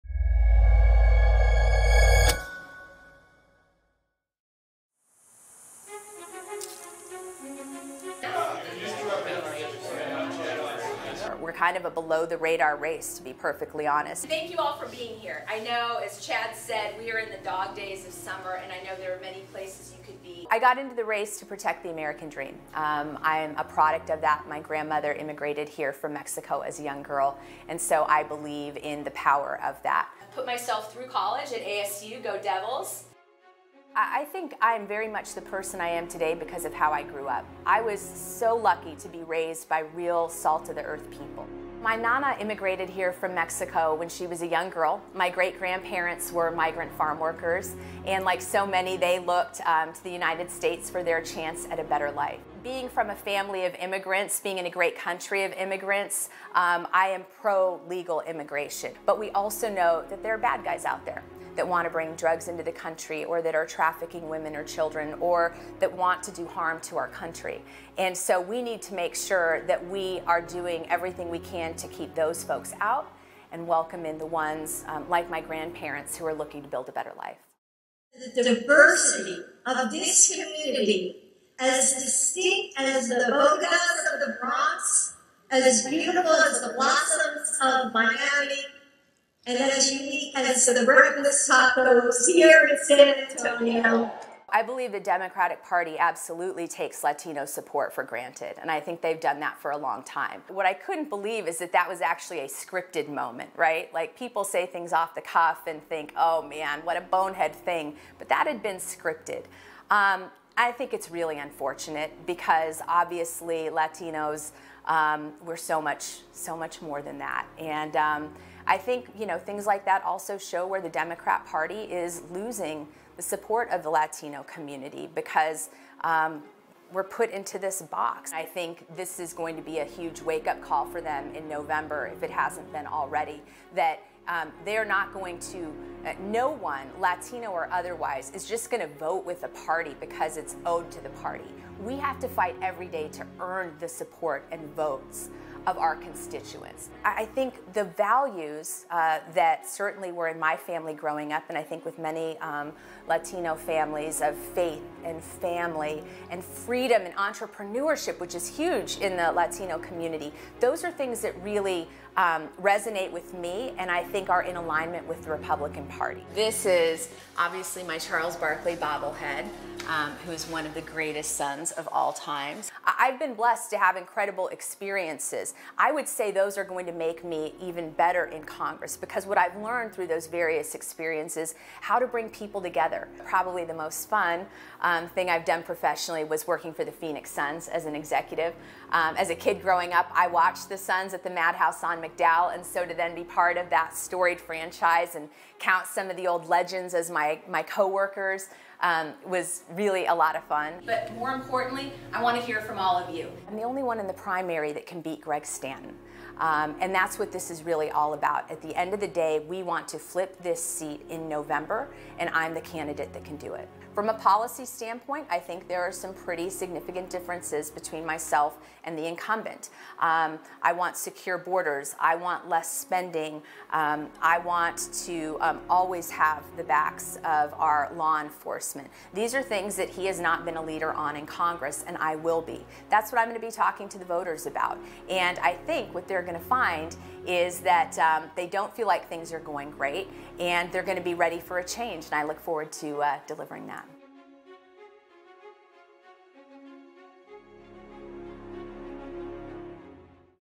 Arizona candidate aims to become state's first Hispanic congresswoman | Digital Originals
Arizona congressional candidate Tanya Contreras Wheeless speaks to Fox Digital ahead of the Republican primary.